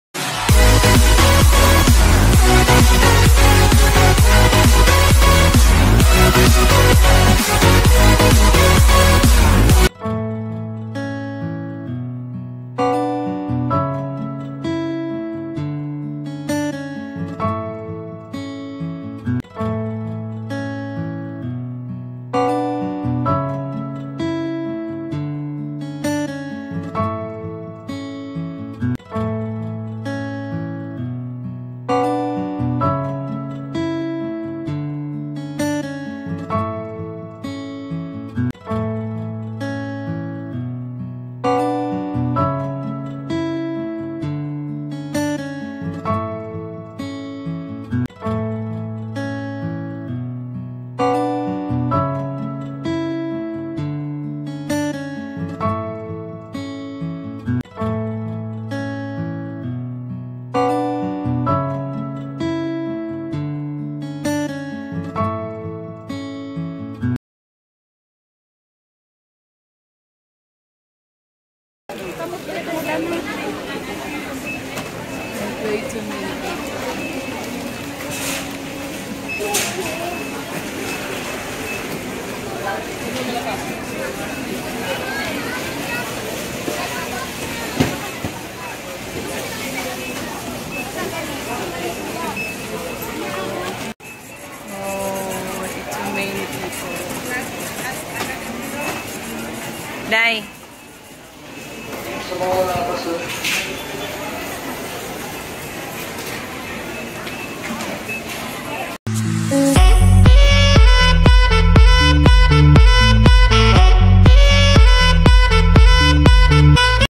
Panic buying sa Gaisano Malaybalay
Ito ang hitsura ng Gaisano Grocery store sa Malaybalay City last  march 17 of this year.. daming taong namili.. at ung ibang mga workers ay hinila na ng grocery department para tumulong sa kanila.